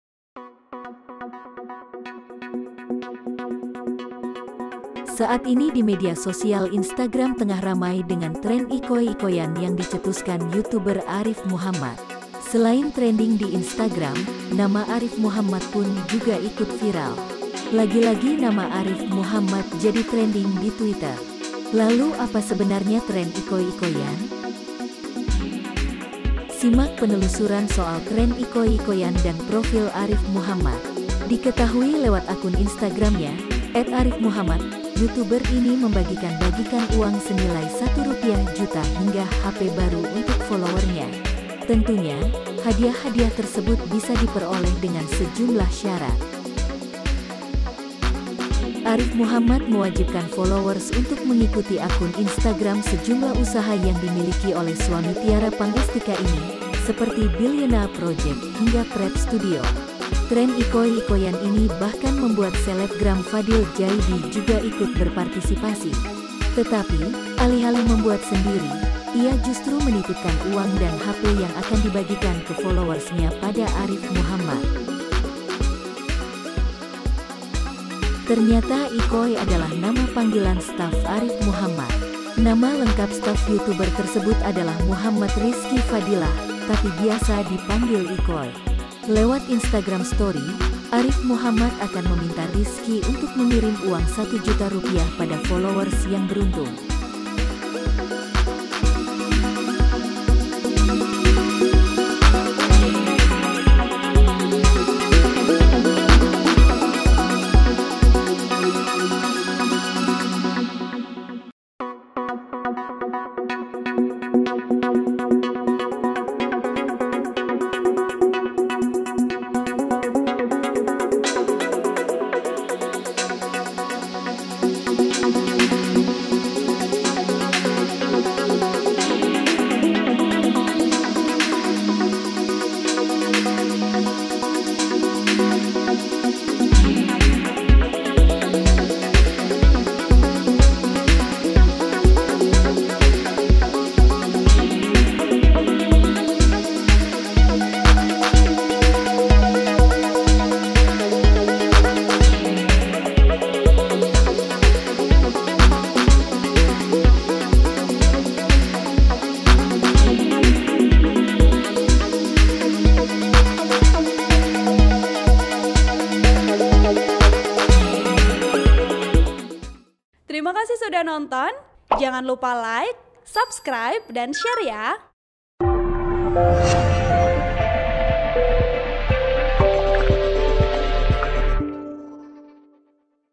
Ramai Ikoy-ikoyan, Apa Artinya? Trending di Instagram setelah Dicetuskan Youtuber Arief Muhammad
Selain trending di Instagram, nama Arief Muhammad pun juga ikut viral.

Edittor: Sandrio